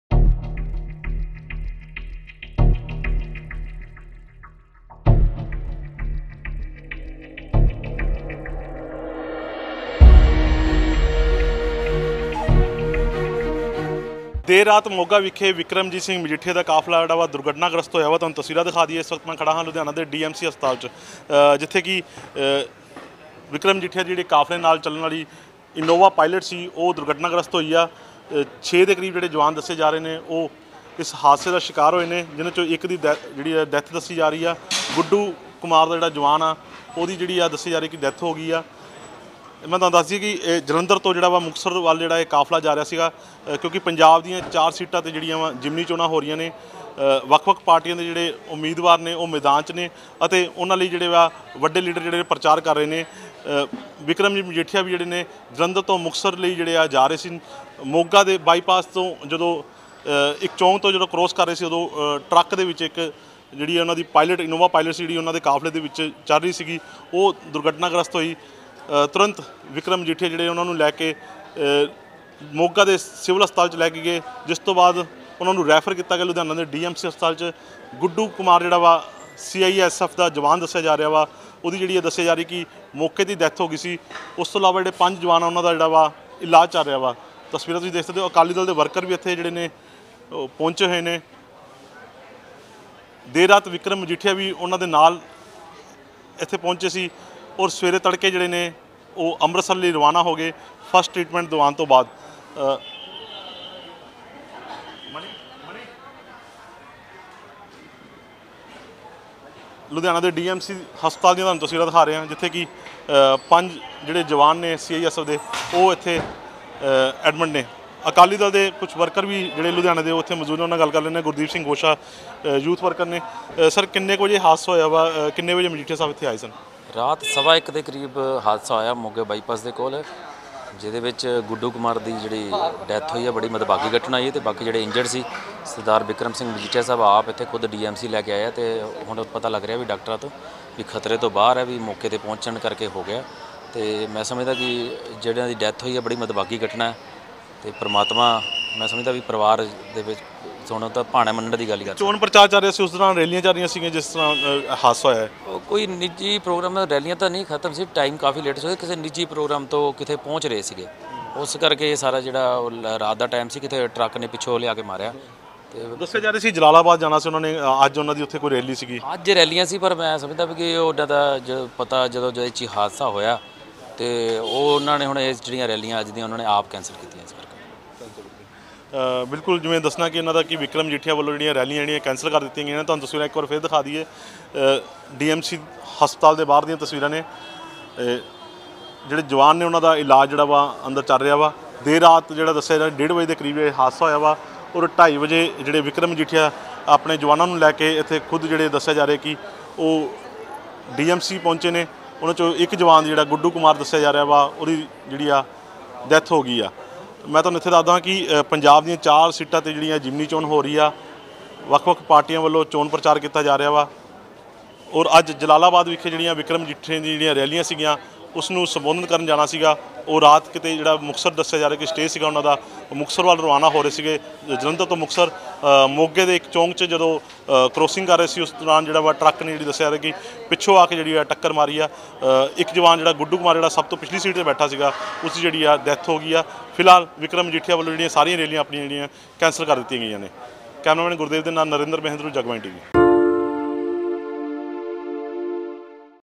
Bikram Majithia की Pilot गाड़ी भयानक हादसे का शिकार , 1 की मौत , 4 जख़्मी
ਬਿਕਰਮ ਮਜੀਠੀਆ ਦੇ ਕਾਫਲੇ ਦੀ ਗੱਡੀ ਹਾਦਸਾਗ੍ਰਸਤ
ਮੋਗਾ ਬਾਈਪਾਸ ਨੇੜੇ ਵਾਪਰਿਆ ਭਿਆਨਕ ਹਾਦਸਾ
ਜਲੰਧਰ ਤੋਂ ਮੁਕਤਸਰ ਜਾ ਰਹੇ ਸੀ ਮਜੀਠੀਆ
ਪਾਇਲਟ ਗੱਡੀ ਦੀ ਟਰੱਕ ਨਾਲ ਟੱਕਰ ਹੋਣ ਤੋਂ ਨਾਲ ਵਾਪਰਿਆ ਹਾਦਸਾ
ਇਨੋਵਾ ਗੱਡੀ 'ਚ ਸਵਾਰ ਸਨ 5 ਸੁਰੱਖਿਆ ਮੁਲਾਜ਼ਮ
ਸੁਰੱਖਿਆ ਮੁਲਾਜ਼ਮਾਂ 'ਚੋਂ 1 ਦੀ ਮੌਤ, 4 ਜ਼ਖਮੀ
ਗੁੱਡੂ ਕੁਮਾਰ ਦੀ ਹੋਈ ਮੌਕੇ 'ਤੇ ਮੌਤ
ਡੀ. ਐੱਮ.ਸੀ. ਹਸਪਤਾਲ ਪਹੁੰਚਾਏ ਗਏ ਜ਼ਖਮੀ